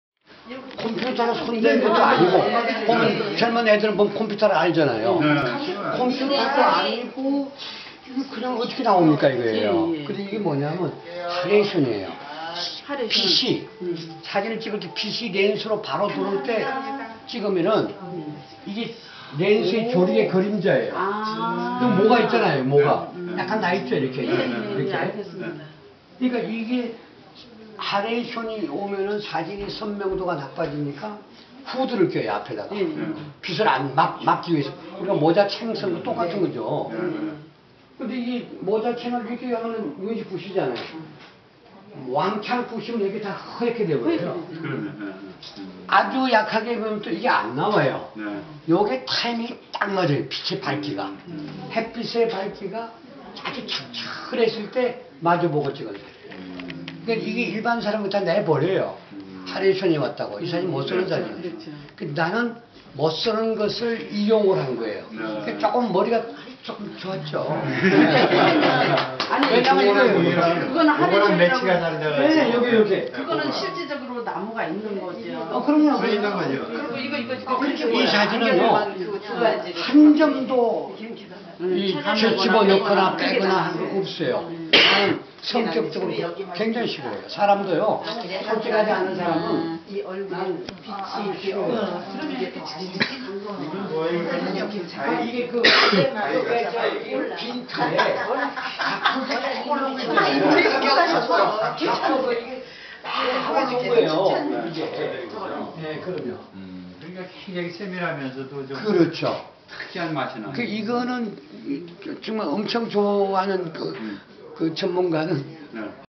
이완교 개인전 ‘피안彼岸 -어둠 저 너머-’ 展 ‘갤러리 서이’에서 개막해 연재2.
북촌에 문을 연 아담한 전통한옥 갤러리 서이(관장 이상미 큐레이터)에서는 원로교수 이완교 초대전 ‘피안彼岸 -어둠 저 너머-’ 展을 열고 5일 오후 개전식을 열었다.
이상미 관장의 사회로 진행된 이날 행사엔 원로교수인 육명심 김대수 작가를 위시하여 가수 김수철 씨, 최광호 김관수 이동선 전민조 최수정 작가 등 많은 인사들이 장내를 가득 메우고 축하의 덕담을 나누었다.

지난 9월4일 시작 된 이번 초대전은 오는 30일까지 계속된다.